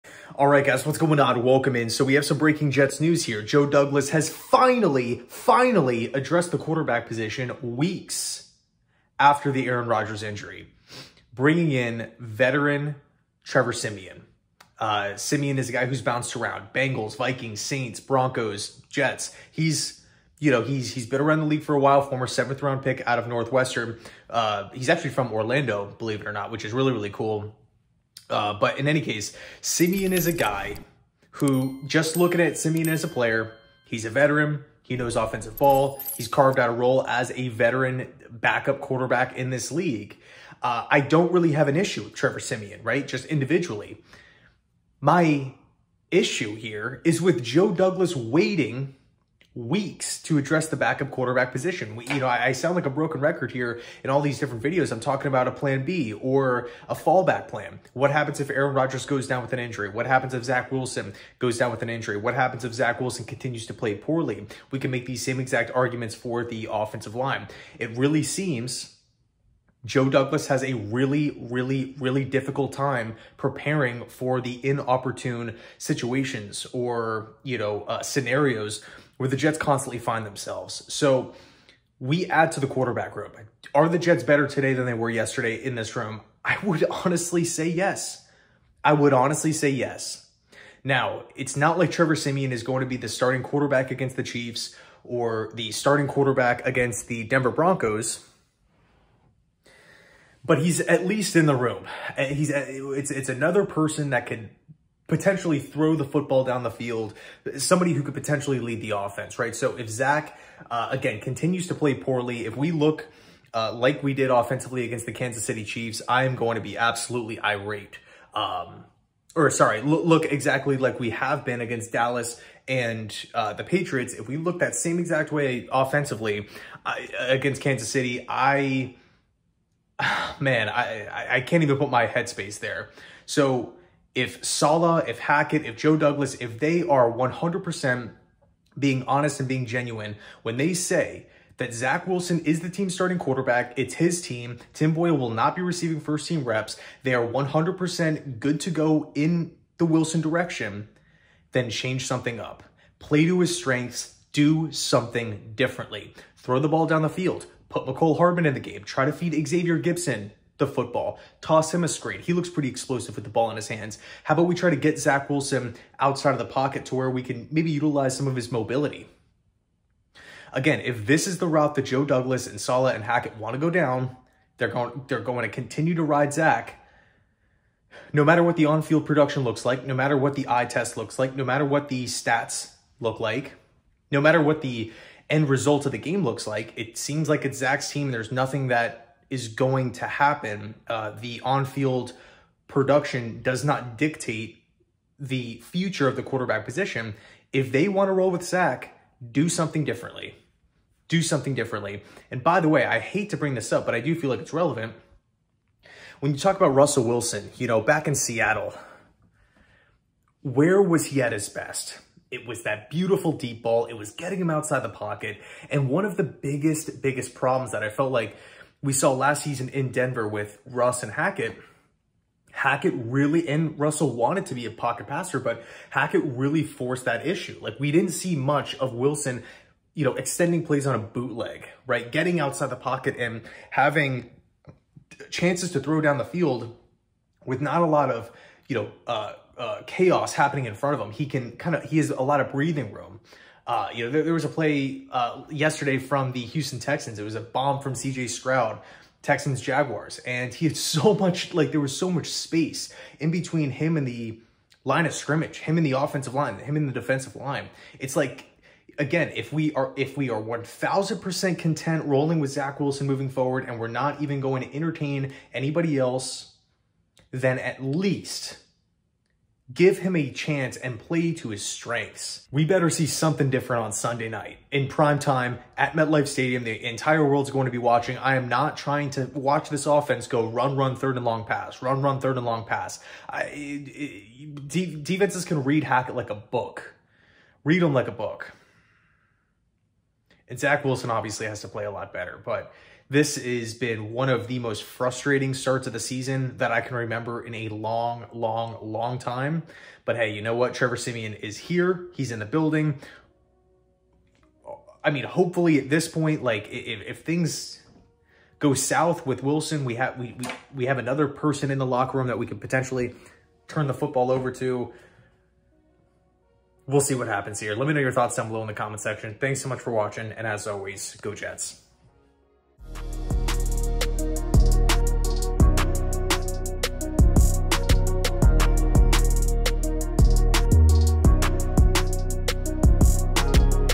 BREAKING: Trevor Siemian SIGNING With The New York Jets | Joe Douglas FINALLY Addresses Quarterback
The New York Jets are signing QB Trevor Siemian according Adam Schefter if he passes his physical. Finally Joe Douglas does something with the QB room. If things go south with Zach Wilson i'm playing Siemian.

Adam Schefter: "Jets are planning to add to their QB room: Veteran QB Trevor Siemian is signing with the Jets, pending a physical, per sources."